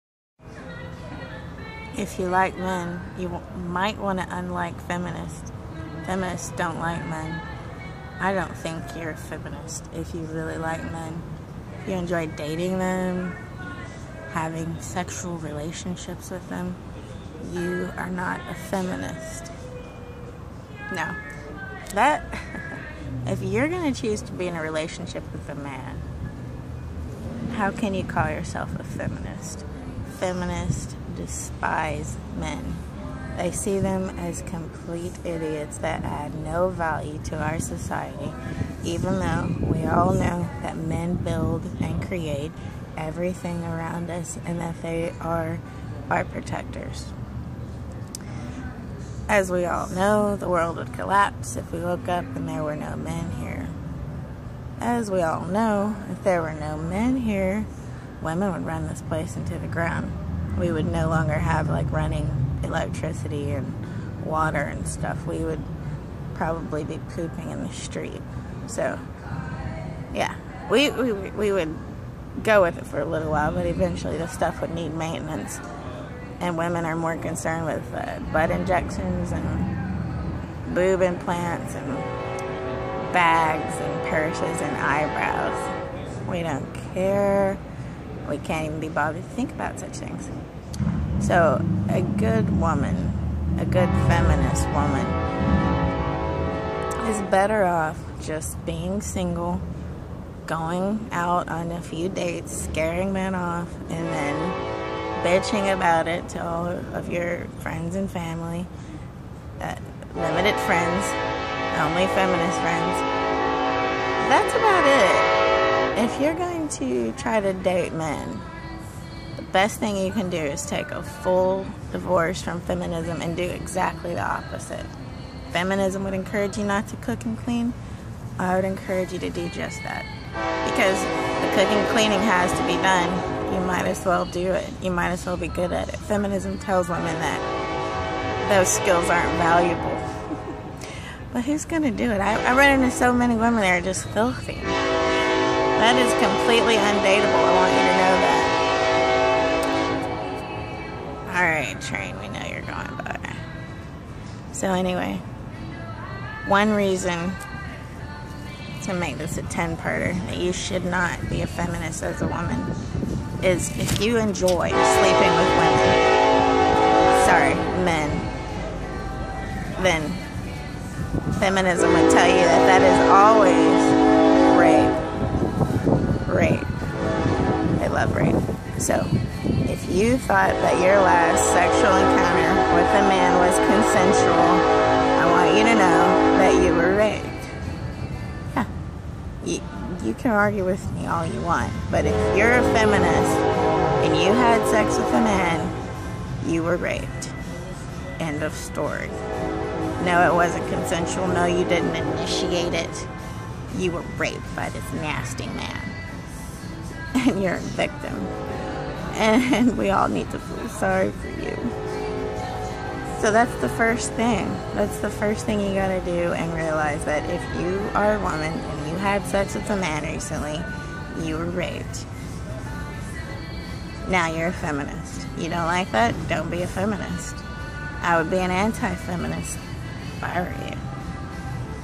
Part 1. Why you're not a feminist.
You're not a feminist you're a biologically functioning woman. Fuck feminist.